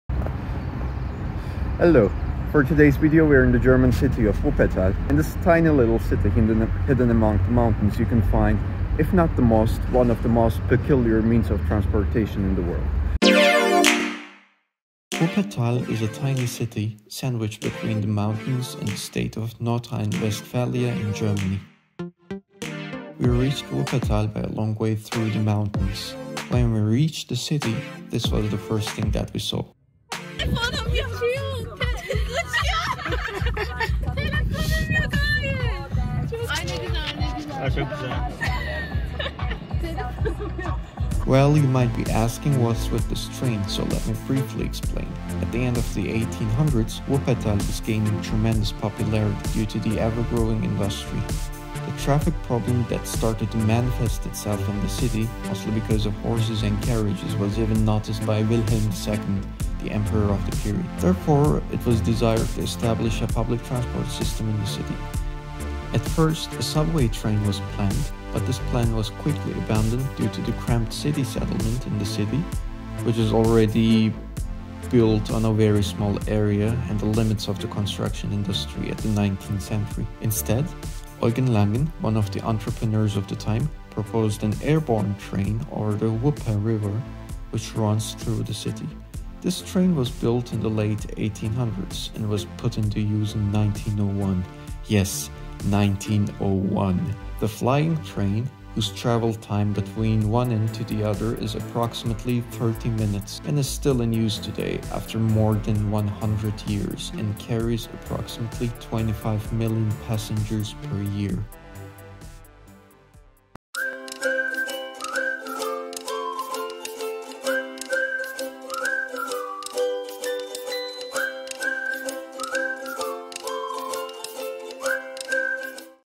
The Century-Old Flying Train In Germany | Wuppertal, Germany - Schwebebahn
Introducing the Schwebebahn in Wuppertal, Germany.